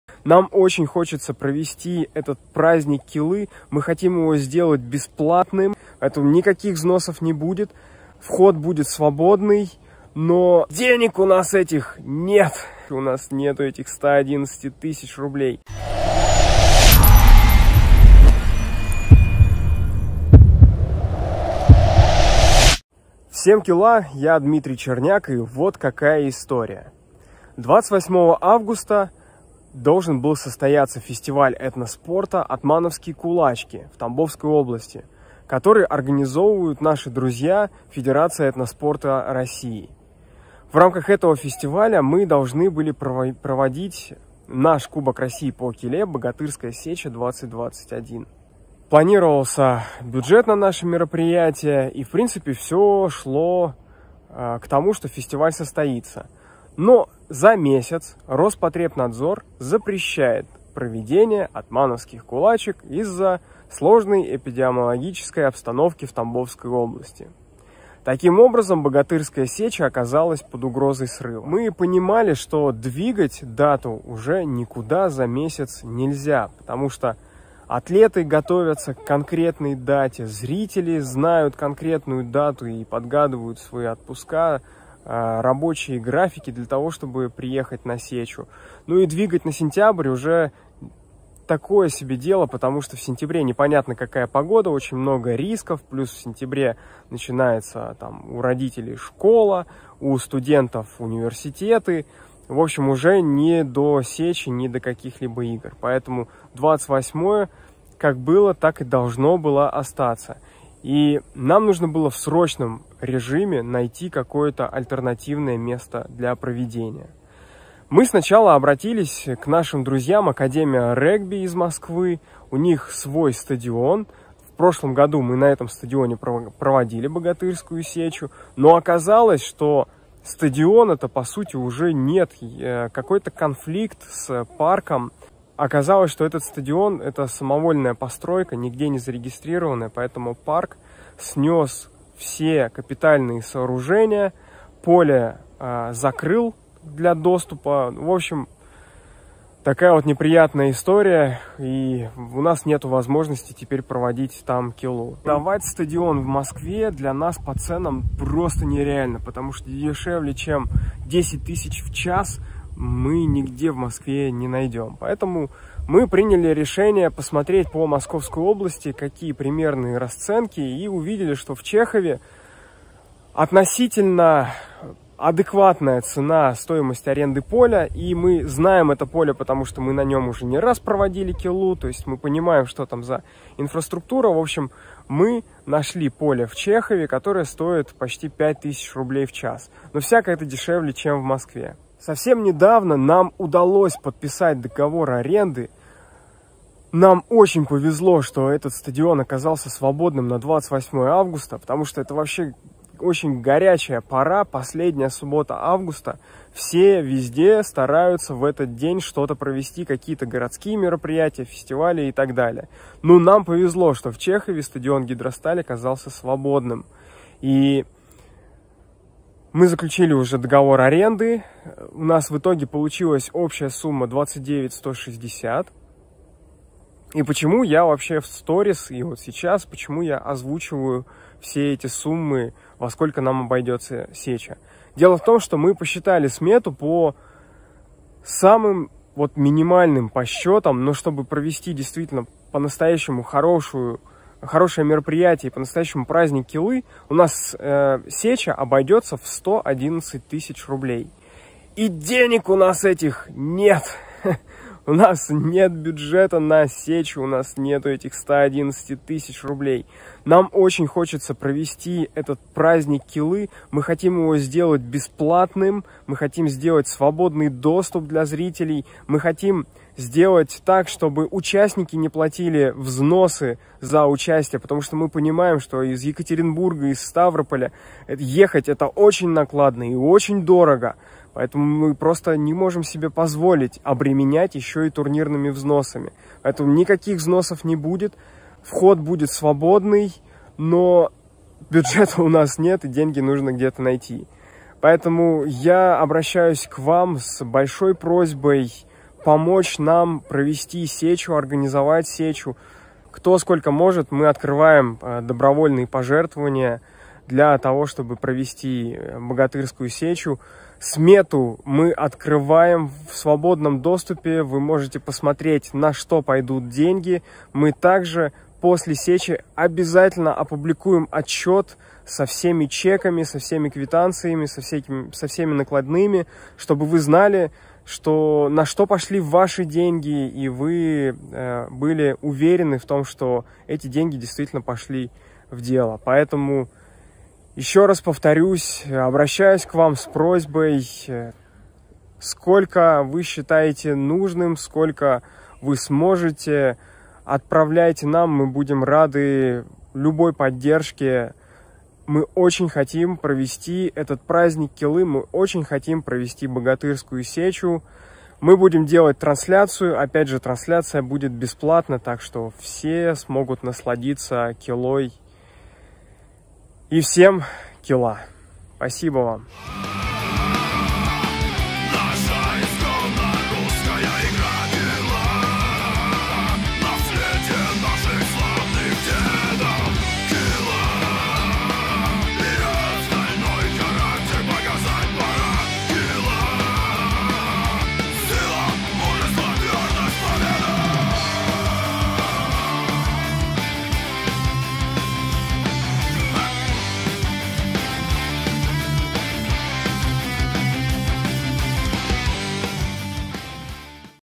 НАМ НУЖНА ВАША ПОМОЩЬ! Как мы остались без бюджета на Богатырскую сечу
В связи с отменой Атмановских кулачек мы оказались в ситуации, когда нам не на что проводить Богатырскую сечу!

Все подробности на видео!

Мы хотим сделать этот праздник килы доступным для всех, поэтому НЕ продаём билеты и НЕ собираем турнирные взносы с участников.
Но для проведения нам необходимо 130 тыс. руб., которых у нас нет.
Раньше мы выкручивались как-то личными финансами, но все личные запасы уже исчерпаны. Поэтому мы обращаемся ко всему сообществу килистов и ко всем неравнодушным к киле людям! Мы подошли уже к той ситуации, что без народной поддержки проведение сечи не осилим.

Для помощи мы открываем возможность внести добровольные пожертвования, чтобы праздник килы состоялся!

(доступны google pay и apple pay)

Либо просто отсканировать камерой смартфона QR-код, который на видео!